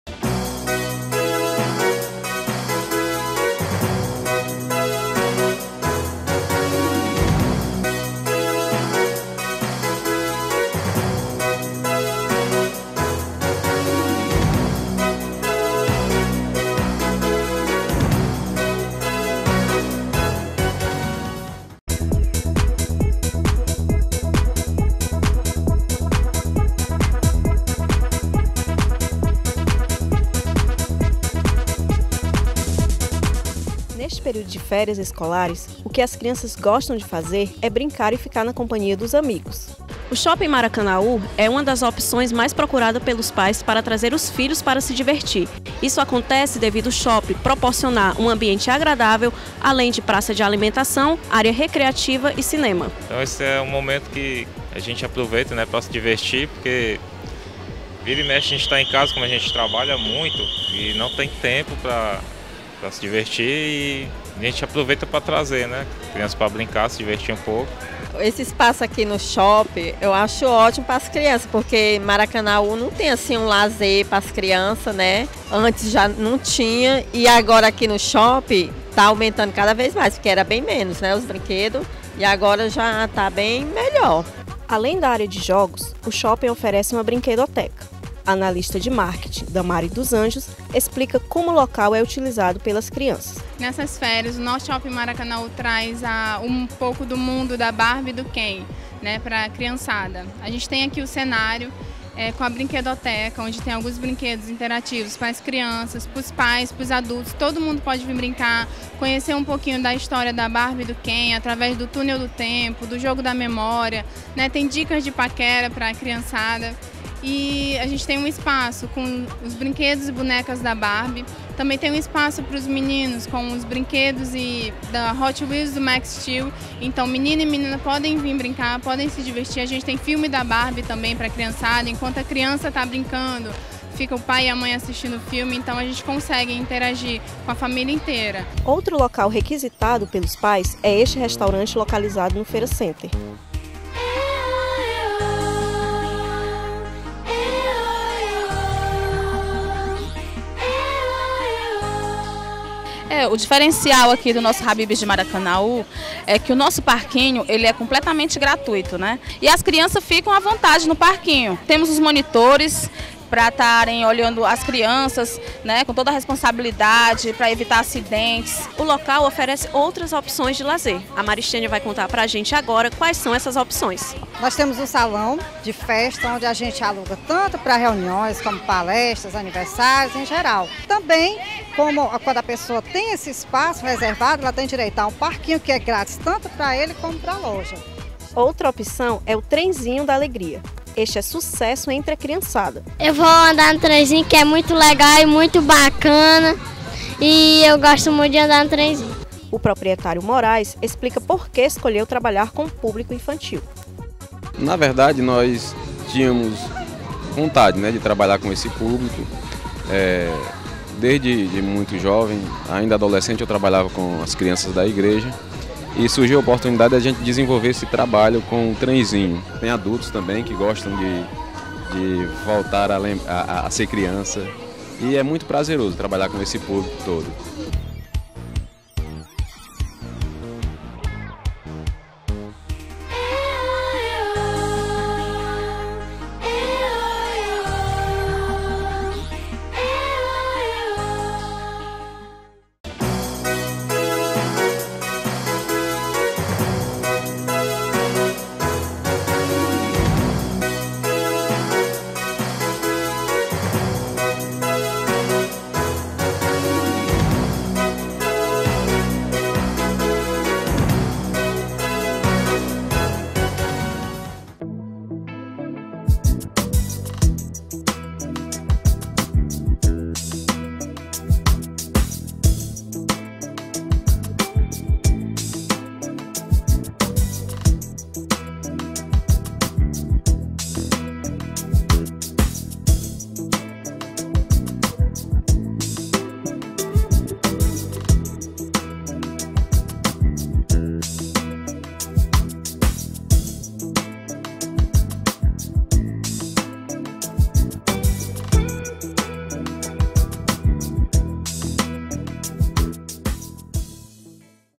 Opções de Lazer no Município de Maracanaú
A equipe do Maracanã Repórter foi conferir as opções de lazer existentes no município de Maracanaú. Diante do que foi visto, as alternativas ainda são poucas em comparação com o porte da nossa cidade.